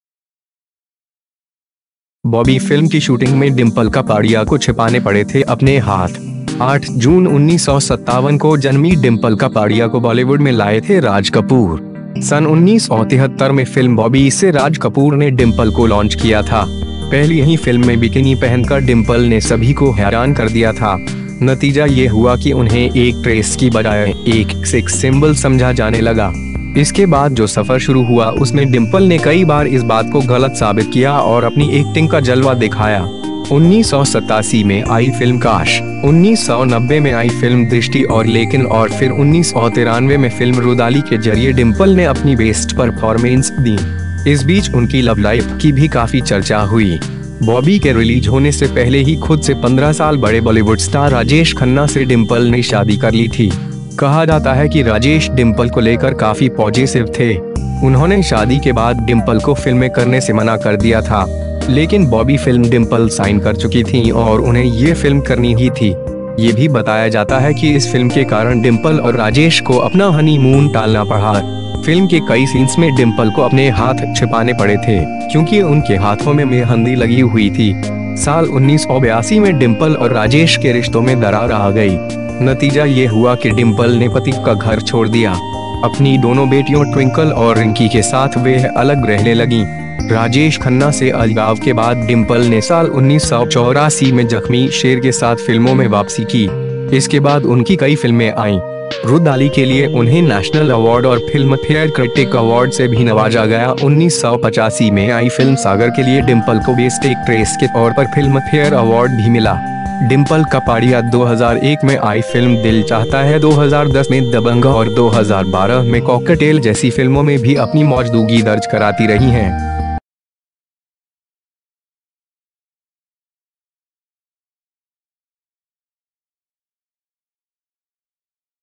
बॉबी फिल्म की शूटिंग में डिंपल कपाड़िया को छिपाने पड़े थे अपने हाथ
8 जून 1957 को जन्मी डिंपल कपाड़िया को बॉलीवुड में लाए थे राज कपूर. सन् 1973 में फिल्म 'बॉबी' से राज कपूर ने डिंपल को लॉन्च किया था. पहली ही फिल्म में बिकिनी पहनकर डिंपल ने सभी को हैरान कर दिया था. नतीजा ये हुआ कि उन्हें एक्ट्रेस की बजाय एक सेक्स सिंबल समझा जाने लगा. इसके बाद जो सफर शुरू हुआ उसमें डिंपल ने कई बार इस बात को गलत साबित किया और अपनी एक्टिंग का जलवा दिखाया.

1987 में आई फिल्म 'काश', 1990 में आई फिल्म 'दृष्टि' और 'लेकिन' और फिर 1993 में फिल्म 'रुदाली' के जरिये डिंपल ने अपनी बेस्ट परफॉर्मेंस दीं.

इस बीच उनकी लवलाइफ की भी काफी चर्चा हुई. बॉबी के रिलीज होने से पहले ही खुद से 15 साल बड़े बॉलीवुड स्टार राजेश खन्ना से डिंपल ने शादी कर ली थी. कहा जाता है कि राजेश डिंपल को लेकर काफी पॉजेसिव थे. उन्होंने शादी के बाद डिंपल को फिल्में करने से मना कर दिया था.

लेकिन बॉबी फिल्म डिंपल साइन कर चुकी थीं और उन्हें ये फिल्म करनी ही थी. ये भी बताया जाता है कि इस फिल्म के कारण डिंपल और राजेश को अपना हनीमून टालना पड़ा. फिल्म के कई सीन्स में डिंपल को अपने हाथ छिपाने पड़े थे, क्योंकि उनके हाथों में मेहंदी लगी हुई थी.

साल 1982 में डिंपल और राजेश के रिश्तों में दरार आ गई. नतीजा ये हुआ कि डिंपल ने पति का घर छोड़ दिया. अपनी दोनों बेटियों ट्विंकल और रिंकी के साथ वह अलग रहने लगीं.

राजेश खन्ना से अलगाव के बाद डिंपल ने साल 1984 में 'जख्मी शेर' के साथ फिल्मों में वापसी की. इसके बाद उनकी कई फिल्में आईं.

'रुदाली' के लिए उन्हें नेशनल अवॉर्ड और फिल्मफेयर क्रिटिक अवॉर्ड से भी नवाजा गया. 1985 में आई फिल्म 'सागर' के लिए डिंपल को बेस्ट एक्ट्रेस के तौर पर फिल्मफेयर अवॉर्ड भी मिला.

डिंपल कपाड़िया 2001 में आई फिल्म 'दिल चाहता है' 2010 में 'दबंग' और 2012 में 'कॉकटेल' जैसी फिल्मों में भी अपनी मौजदूगी दर्ज कराती रही हैं.